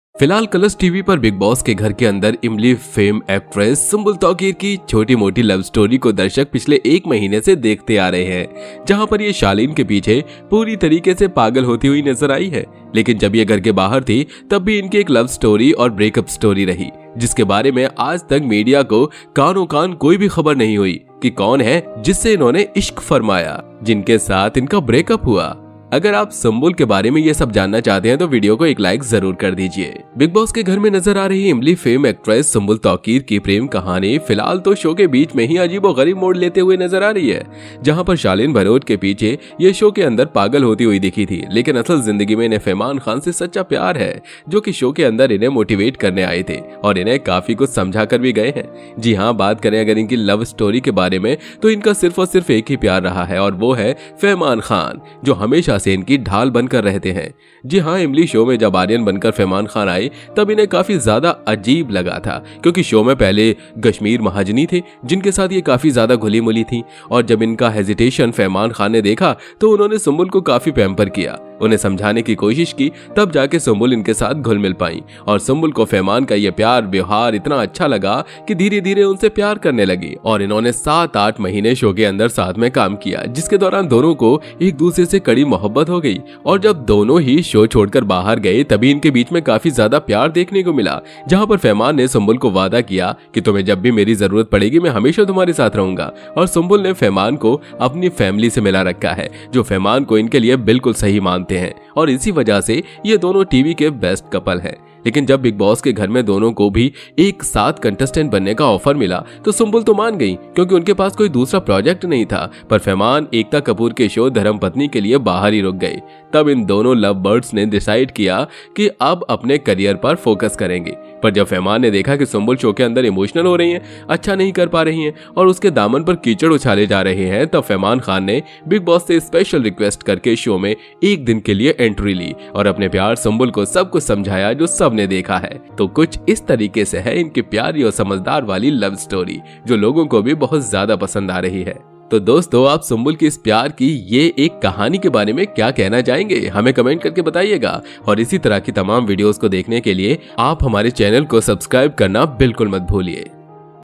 इमली सीरियल से मशहूर हुई अभिनेत्री सुम्बुल की जानिए लव स्टोरी और ब्रेकअप स्टोरी.
Know the love story and breakup story of actress Sumbul who became famous with Imlie serial.